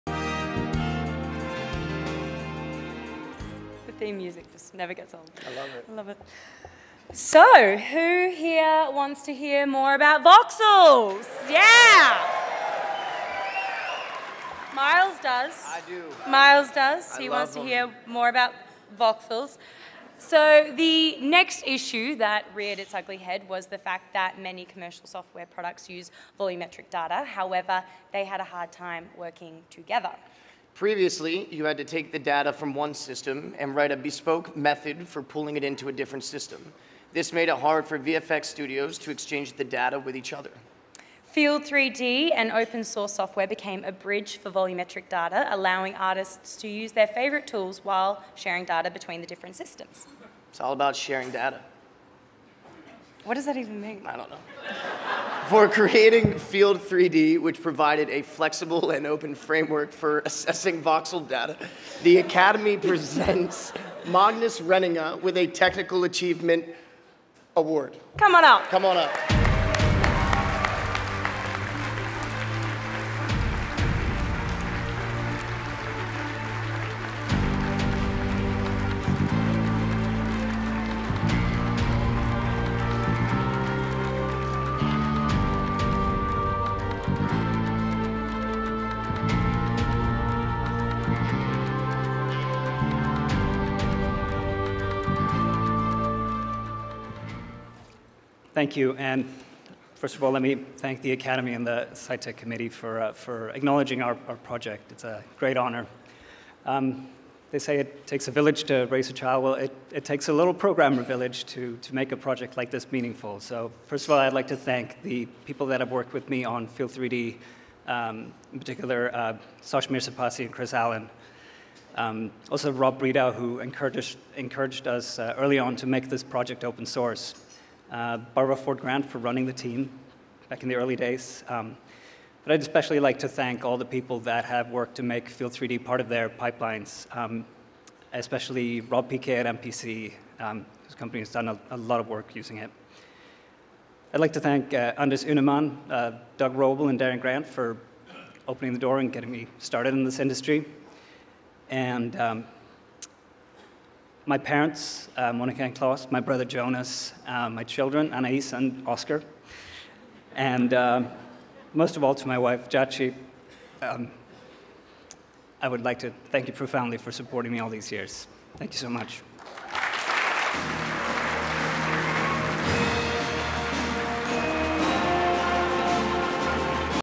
2014 Sci-Tech Awards: Magnus Wrenninge
Margot Robbie and Miles Teller present a Technical Achievement Award to Magnus Wrenninge for leading the design and development of Field3D. From the Academy's Scientific & Technical Awards held on February 7, 2015 at the Beverly Wilshire.